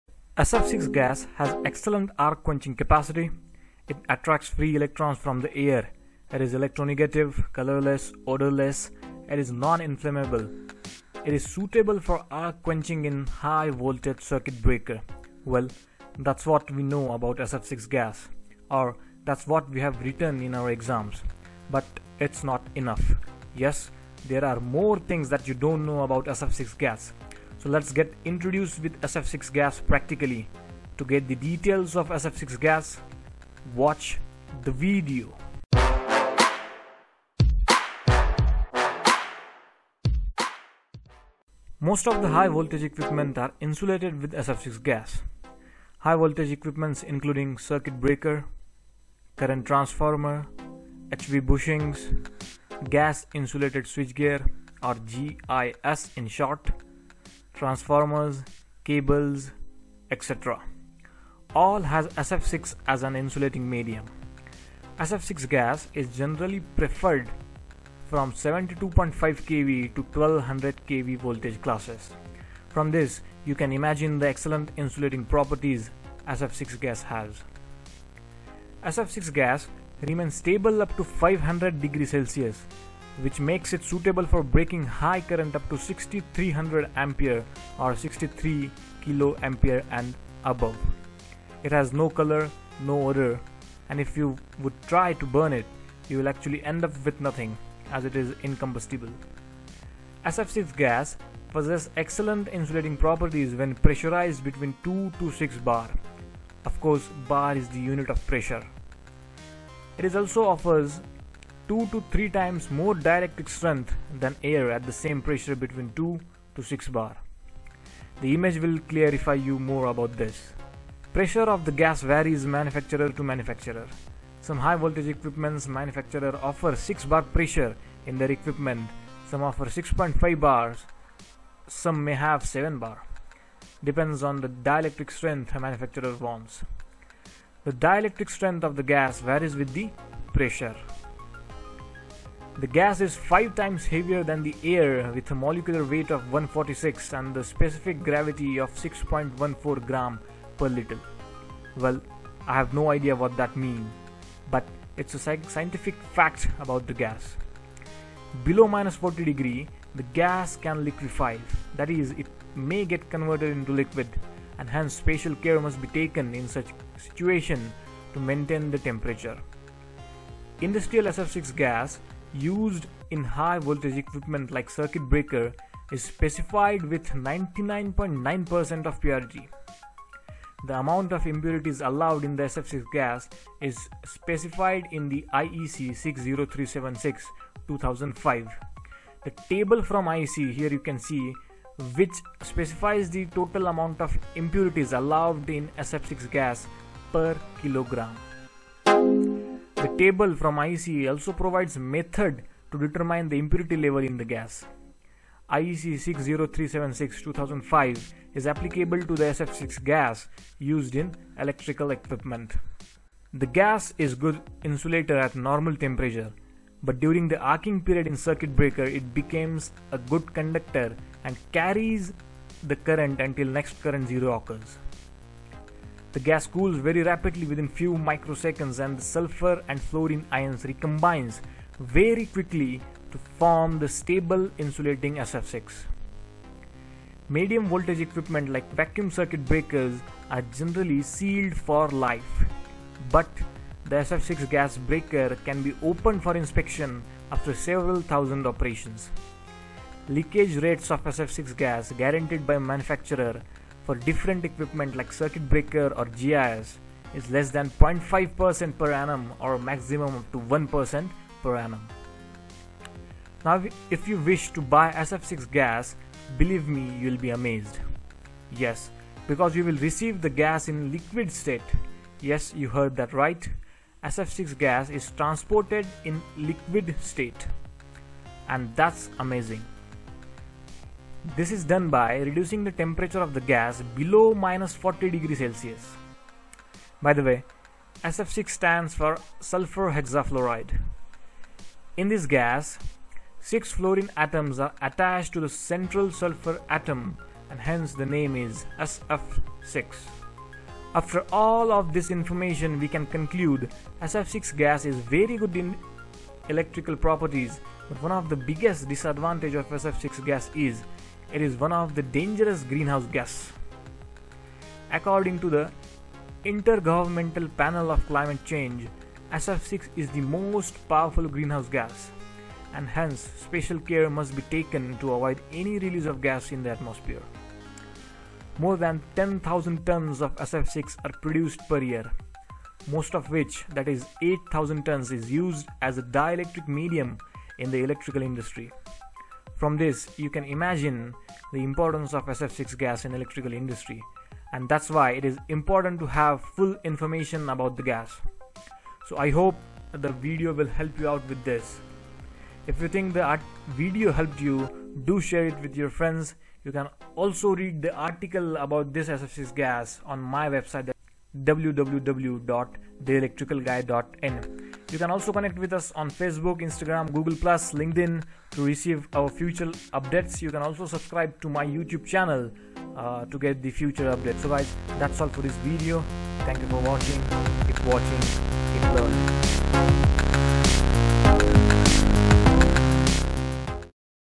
The things you don't know about SF6 gas | TheElectricalGuy
Know the interesting things about the most used gas in electrical industry i.e. SF6 gas.

10,000 tons of SF6 gas is produced per year, out of that 8,000 tons gas is used in electrical industries. And, hence it is very important to have detail info about SF6 gas but, we have very few. Know beyond what we have written in the exams about sf6 gas. Get introduce with SF6 gas practically only by watching this video.